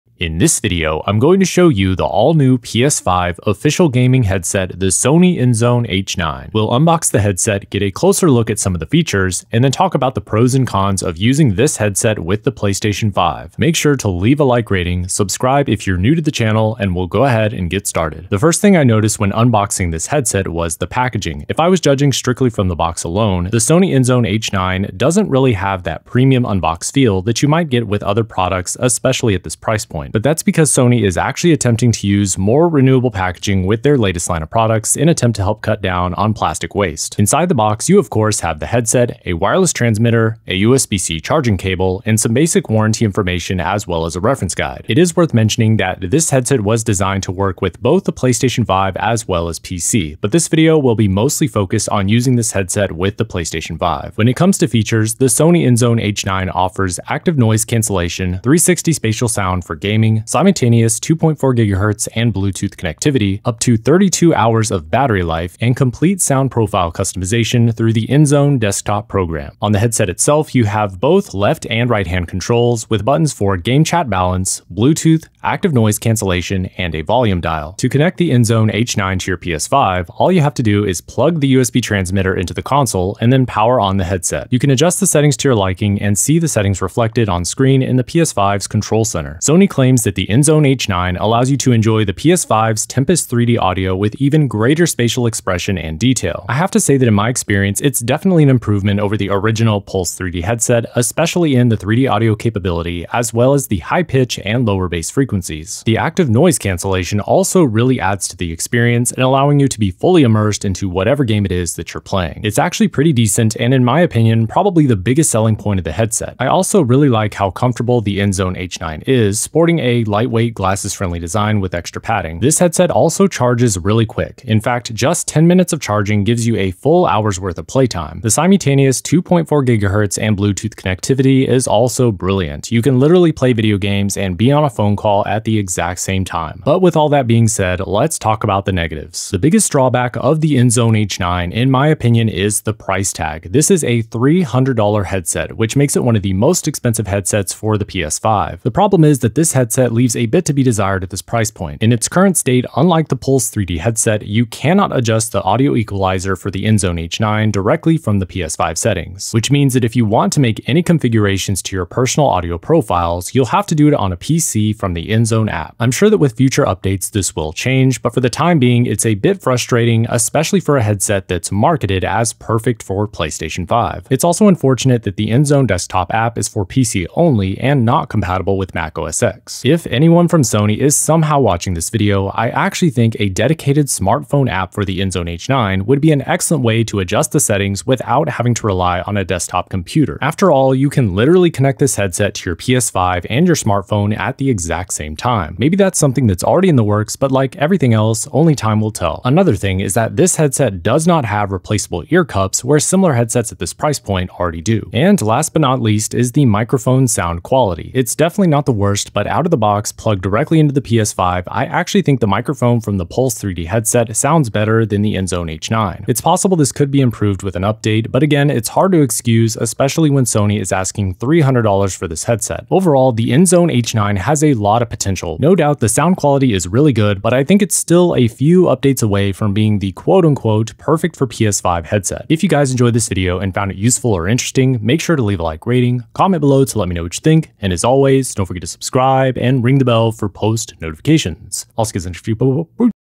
I Got The New PS5 Official Headset
Here's the new PS5 official Sony headset! I got the new Playstation 5 Sony INZONE H9, The perfect for PS5 headset that changes everything.. Is it better than Sony Pulse 3D for Playstation 5? Here's the truth. In this video, I unbox & review the all new Sony PS5 INZONE H9 gaming headset! The "perfect for PS5" official gaming headset from Sony.. Built with Tempest 3D Spatial Sound, Active Noise Cancelation, & more! .. Here's how to connect the Sony PS5 headset to your Playstation 5 & change settings with the new INZONE Desktop App. You can use the official PS5 headset with PC also!.. Is this new gaming headset for PS5 worth it? Find out in this video

Sony INZONE H9 Gaming Headset:

As an associate, I may earn from qualifying purchases.